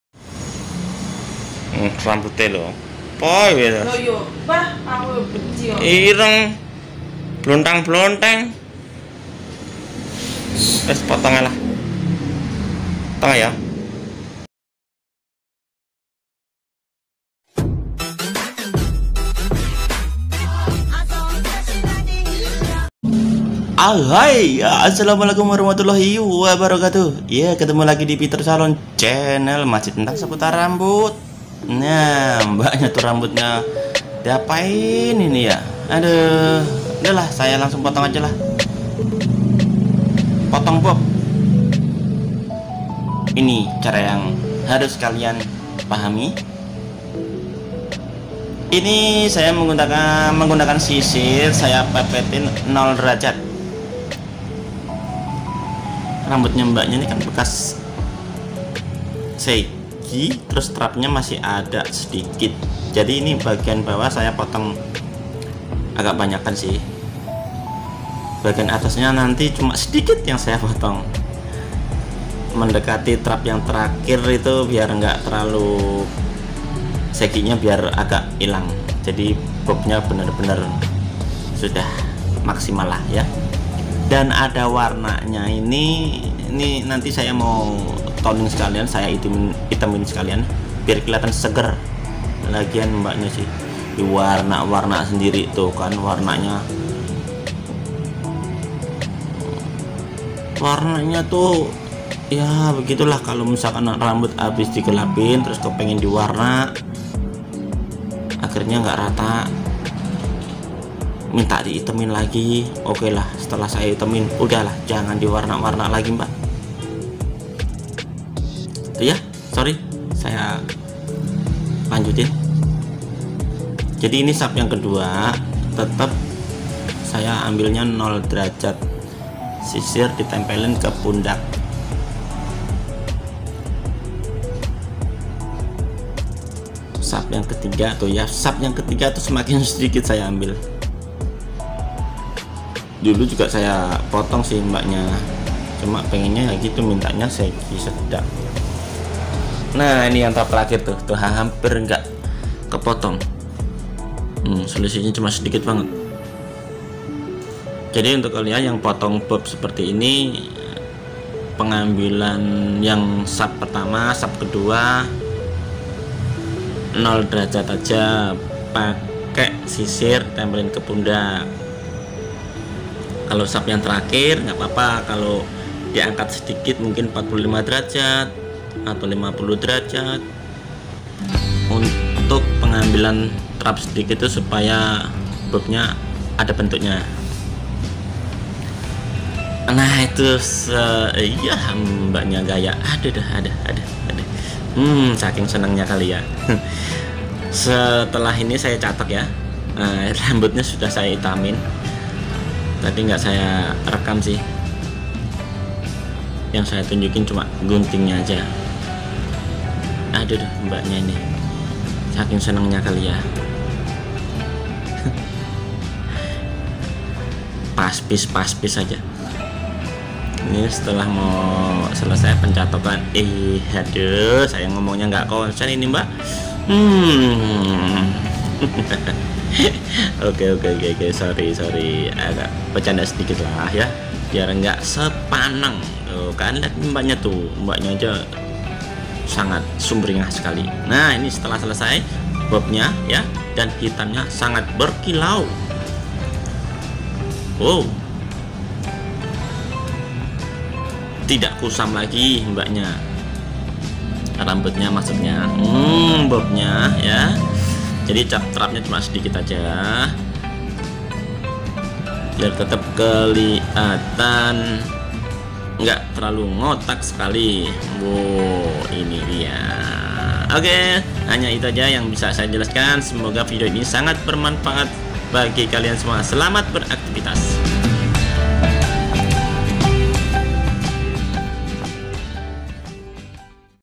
gunting model bob nol Drajad/tanpa pengangkatan
simple mudah diatur dan cocok untuk face apa aja itulah model bob yg selalu diminati kaum perempuan
ini saya gunting model bob nol Drajad/tanpa pengangkatan
ooo iya...rambut mbaknya saya Toning/hitamkan sekalian biar lebih glowing

simak videonya sampe abis

video yg lain